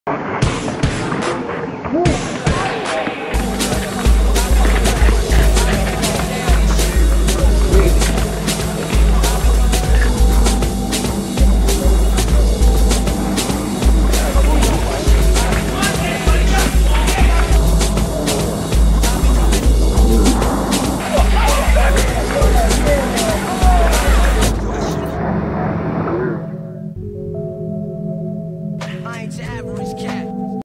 Dorf with the Delly Defense/ Silva strong finish MAYHEM99
Mayhem'99 Basketball Tournament
Lakewood, Ohio
edit by Brian Foox